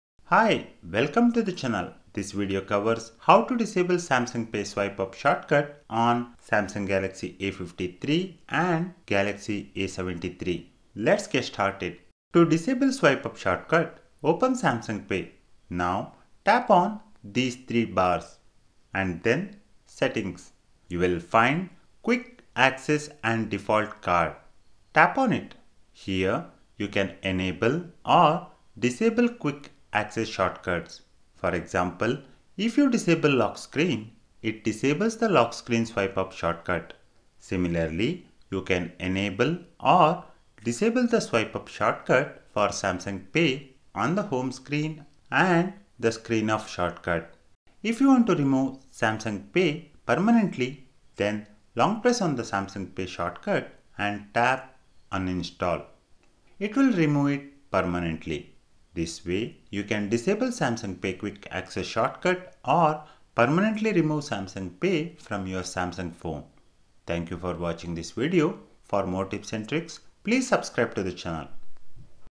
Samsung Galaxy A53/A73: How to disable Samsung Pay Swipe up shortcut or remove permanently
Video on how to disable Samsung Pay Swipe up shortcut or uninstall Samsung Pay from the phone.